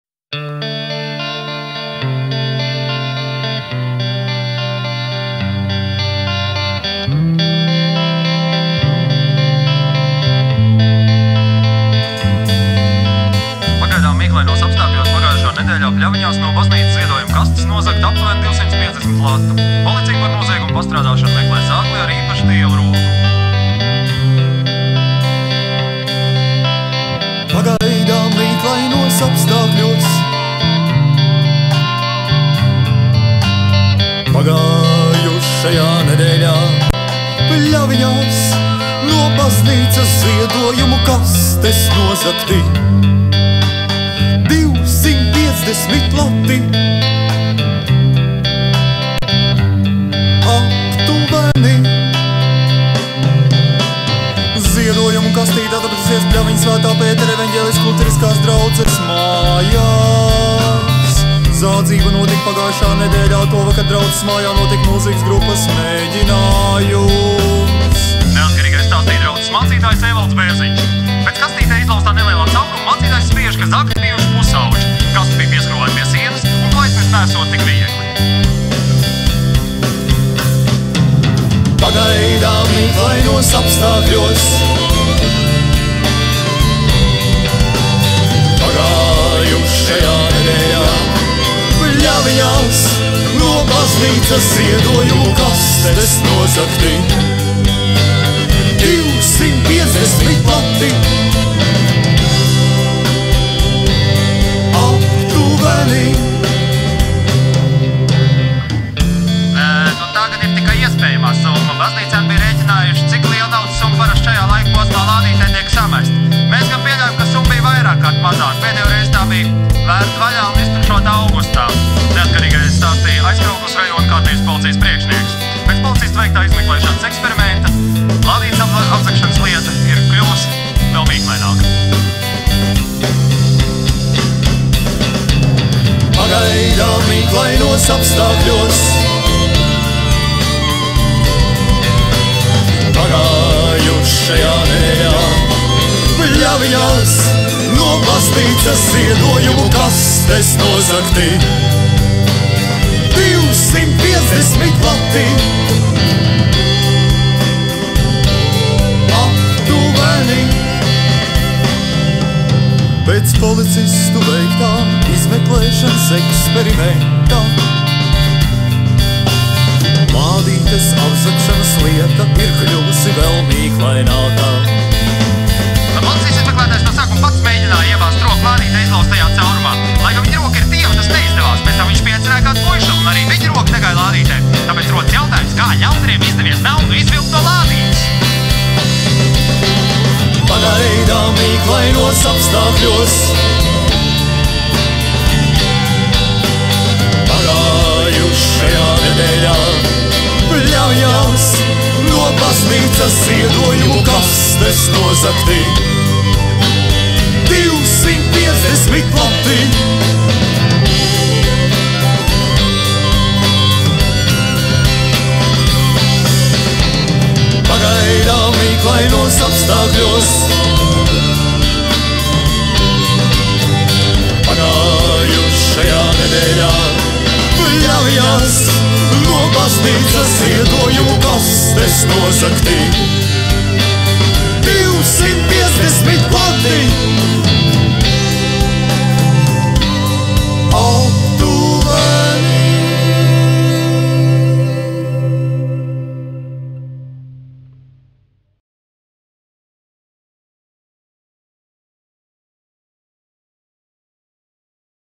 Suņa stunda - Pļaviņas
Pagaidām mīklainos apstākļos pagājušajā nedēļā Pļaviņās
no baznīcas ziedojumu kastes nozagti aptuveni 250 latu.
Policija par nozieguma pastrādāšanu meklē zagli ar
īpaši tievu roku.

Piedz.
   Pagaidām mīklainos apstākļos
   pagājušajā nedēļā / Pļaviņās
   no baznīcas ziedojumu kastes  nozagti
   250 lati
   aptuveni

Ziedojumu kastīte atradusies Pļaviņu Sv. Pētera
evaņģēliski luteriskās draudzes mājā.
Tovakar draudzes mājā notika mūzikas grupas mēģinājums,

Neatkarīgajai
stāstīja draudzes mācītājs Ēvalds Bērziņš.
Pēc kastītē izlauztā nelielā cauruma mācītājs spriež,
ka zagļi bijuši pusaudži. Kaste bija pieskrūvēta pie
sienas, un to aiznest neesot tik viegli.

Piedz.

"Tā gan ir tikai iespējamā summa. Baznīcēni bija
rēķinājuši, cik liela naudas summa parasti šajā laikā
posmā lādītē tiek samesta. Mēs gan pieļaujam, ka summa
bija vairākkārt mazāka. Pēdējo reizi tā bija vērta vaļā
un iztukšota augustā," Neatkarīgajai sacīja Aizkraukles
rajona Kārtības policijas priekšnieks.

Piedz.

Pēc policistu veiktā izmeklēšanas eksperimenta lādītes
apzagšanas lieta kļuvusi vēl mīklaināka.
"Policijas izmeklētājs no sākuma pats mēģināja iebāzt
roku lādītē izlauztajā caurumā,
lai gan viņa roka ir tieva, tas neizdevās.
Pēc tam viņš paaicināja kādu puišeli, un arī viņa roka
negāja lādītē,

tāpēc rodas jautājums – kā ļaundariem izdevies naudu
izvilkt no lādītes,"

Piedz.
2X